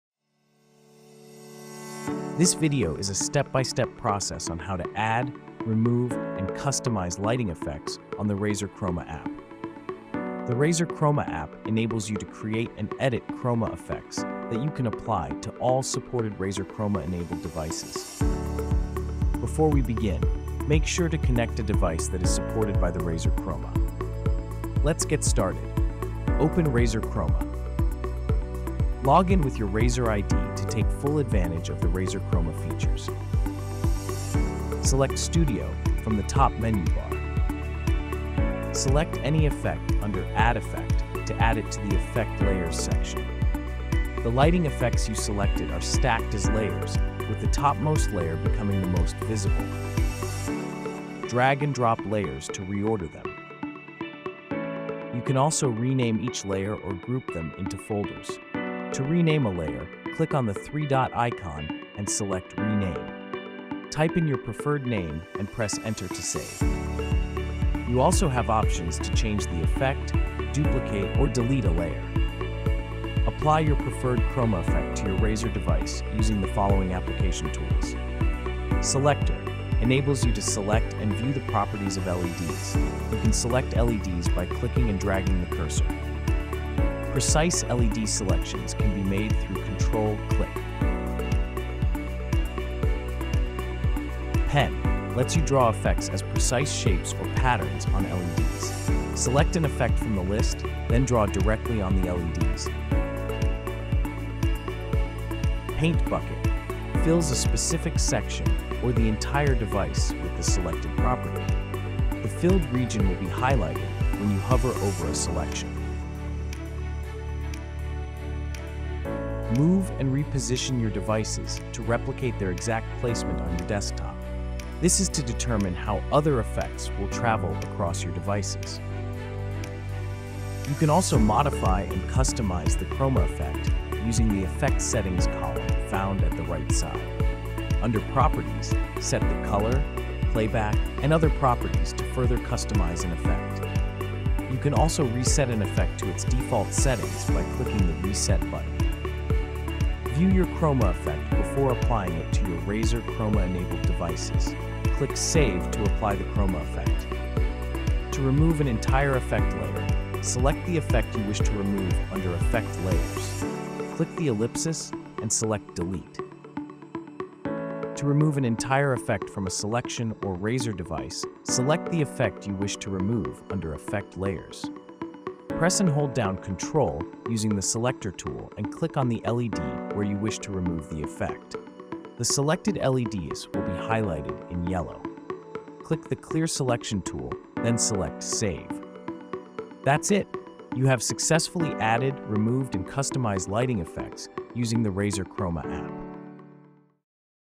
How to add, remove and customize lighting effects on Razer Chroma app
This video will show you how to add, remove and customize lighting effects on the Razer Chroma App.